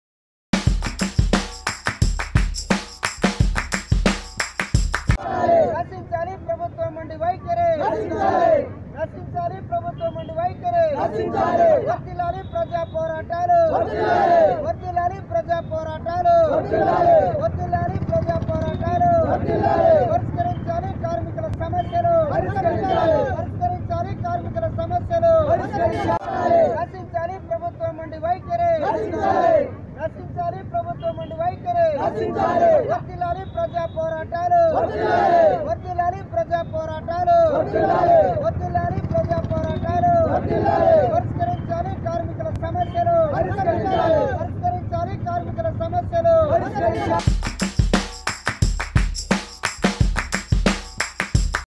కేసీఆర్ కు బిగిస్తున్న ఉచ్చు, ఆర్టీసీ సమ్మెకు సిపిఐ సపోర్ట్ | CPI Supports TSRTC Strike | Ashwathama
కేసీఆర్ కు బిగిస్తున్న ఉచ్చు, ఆర్టీసీ సమ్మెకు సిపిఐ సపోర్ట్ | CPI Supports TSRTC Strike | Ashwathama Reddy | Telugu Breaking